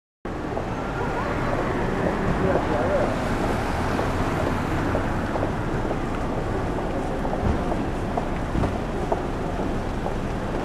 (東武)新越谷駅前東口&(JR)南越谷駅前南口時計台
どちらも埼玉/越谷市,南越谷1丁目にある駅で接続駅になっています

(東武)新越谷
「東武スカイツリーライン」の愛称区間に含まれます。
駅番号はTS 20。
JR方面は東口になります

(JR)南越谷
(東武)方面は南口になります

(東武)新越谷東口
(JR)南越谷南口
共同駅前です